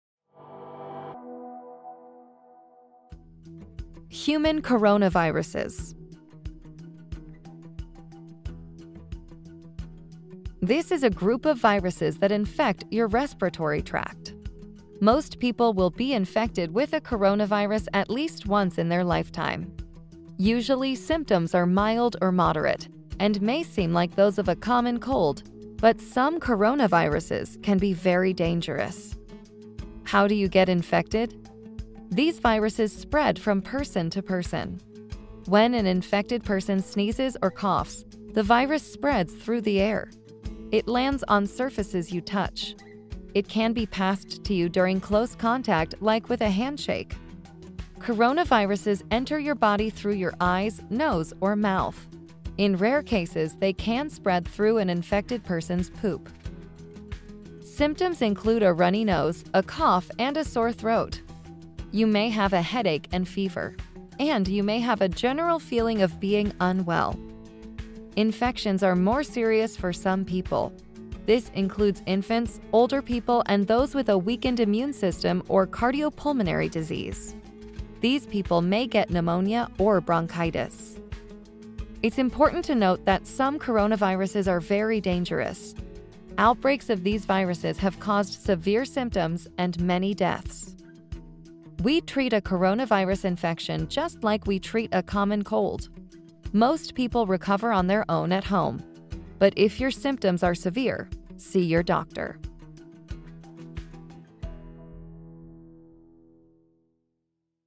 Human Coronaviruses
This is a group of viruses that infect your respiratory tract. Most people will be infected with a coronovirus at least once in their life. Usually, symptoms are mild or moderate, and may seem like those of a common cold. But some coronaviruses can be very dangerous.